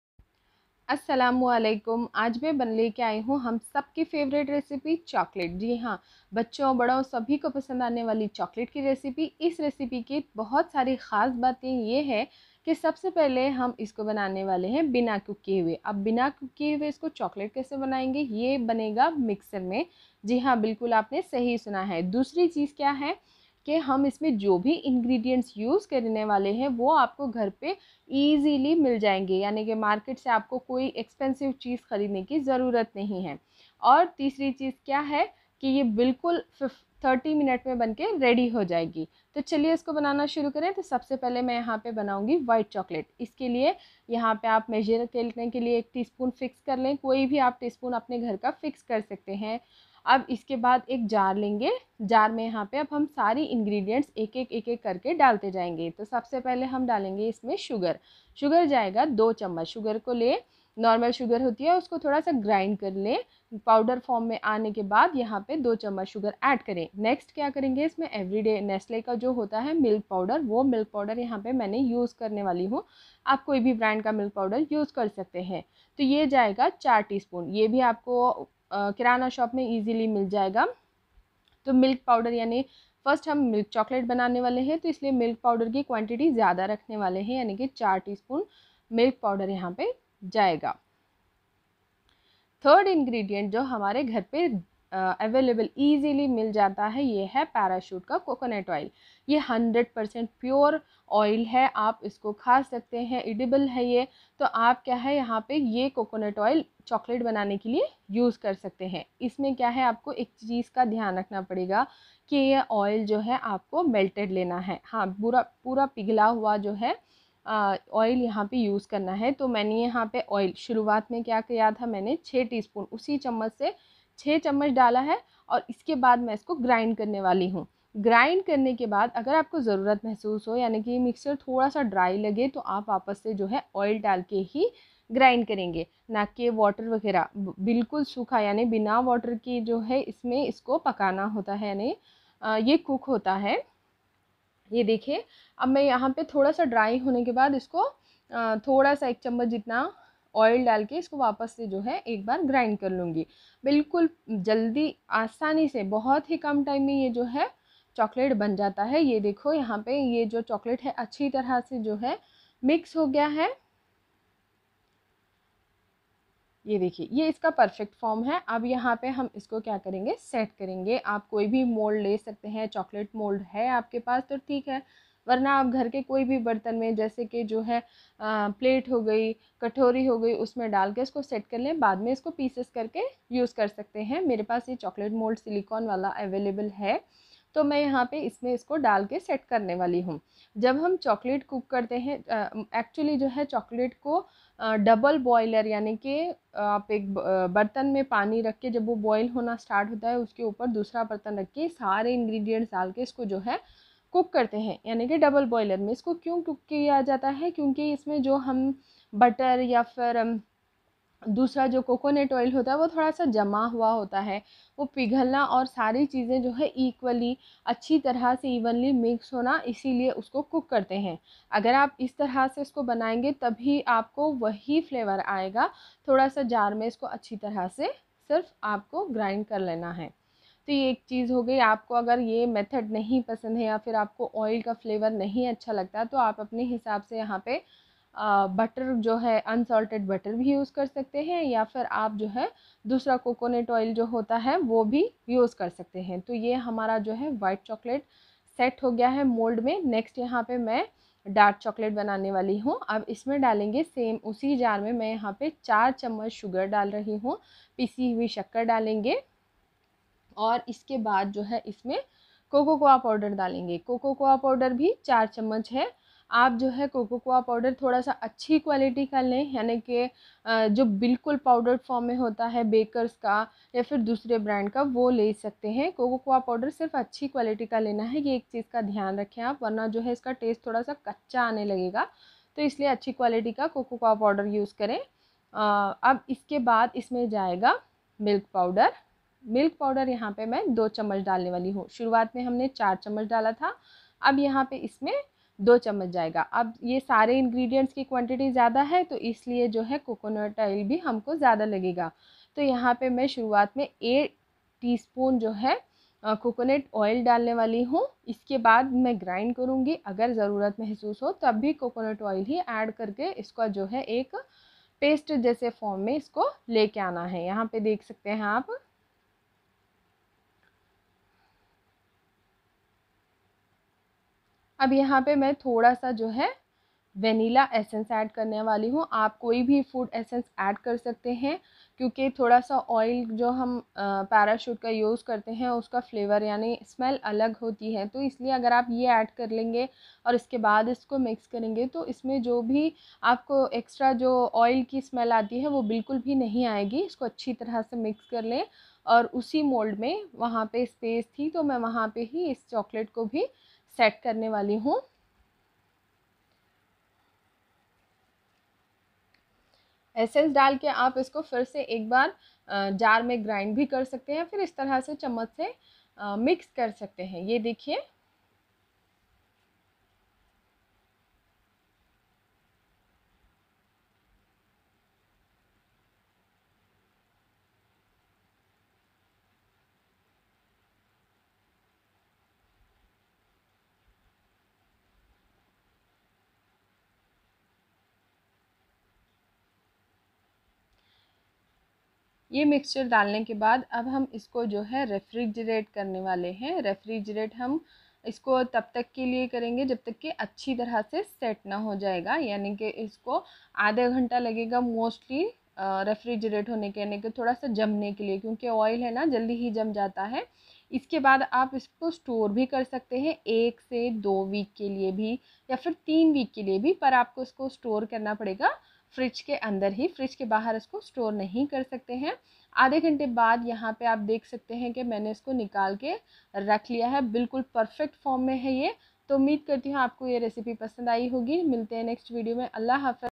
No cooking chocolate Recipe only with 3 main ingredients |easy chocolate recipe in 10 minutes
ingredients

for white chocolate

sugar- 2tsp
milk powder- 4tsp

for milk chocolate
sugar- 4tsp
milk powder- 2tsp
cocoa powder-4tsp